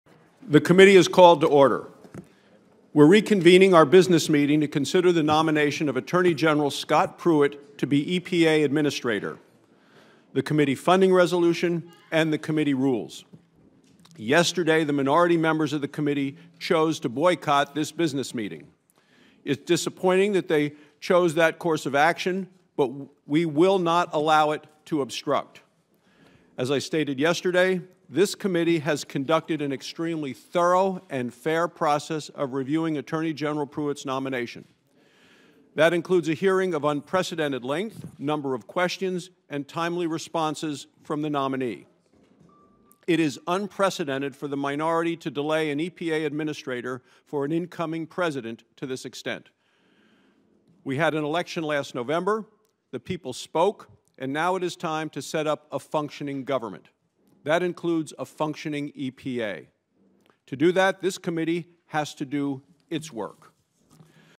Chairman Barrasso's Opening Statement at a Committee Business Meeting
U.S. Senator John Barrasso (R-WY), chairman of the Senate Committee on Environment and Public Works (EPW), delivered the following remarks at a committee business meeting to consider the nomination of Oklahoma Attorney General Scott Pruitt to be administrator of the Environmental Protection Agency. The remarks came in response to the committee’s Democrats boycotting the business meeting. During the meeting, the committee voted to suspend its rules and move Attorney General Pruitt’s nomination forward. (February 2, 2017)